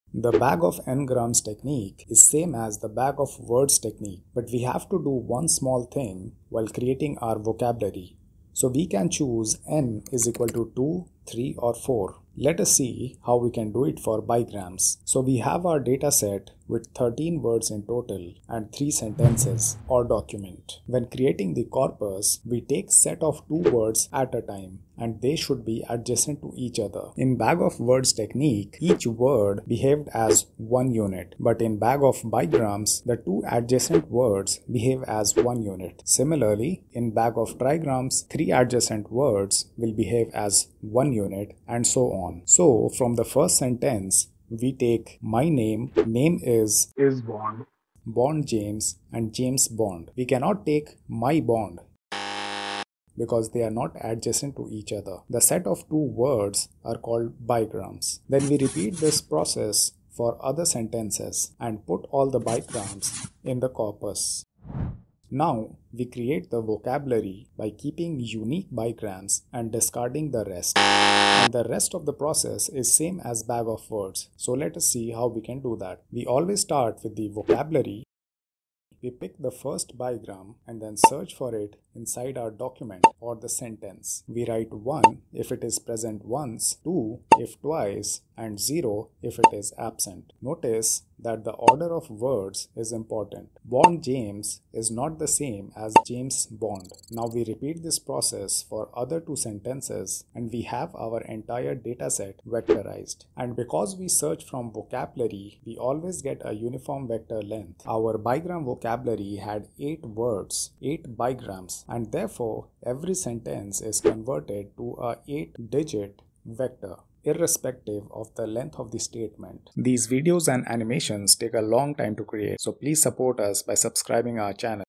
Bag of n-grams in 120 seconds !! NLP Text vectorization
Bag of n-grams is a text vectorization technique coomonly used in NLP. Text vectorization converts text to numbers which can be processed by Machine learning and deep learning algorithms.